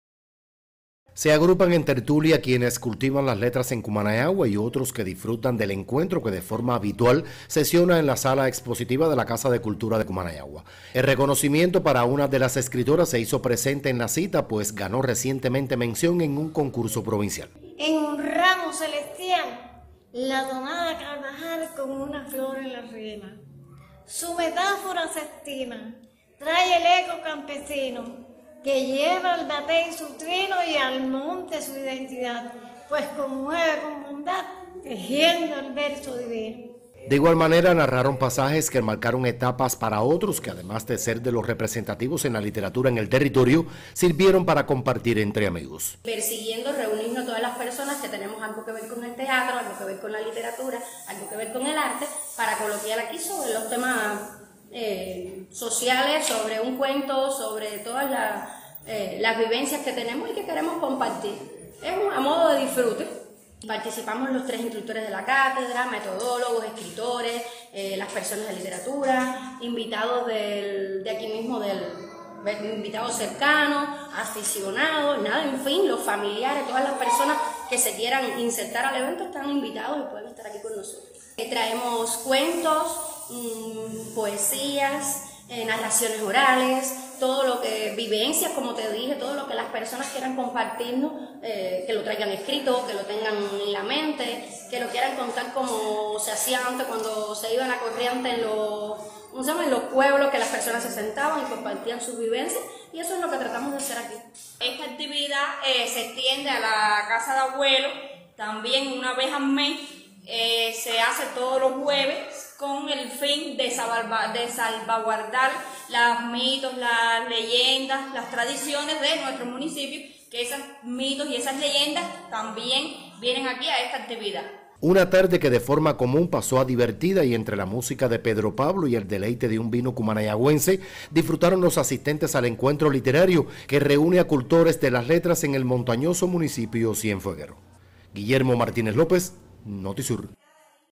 Celebran encuentro literario en Cumanayagua
Escritores, teatristas y público en general participaron en el encuentro literario que se realiza cada mes en la Casa de la Cultura Habarimao, de Cumanayagua.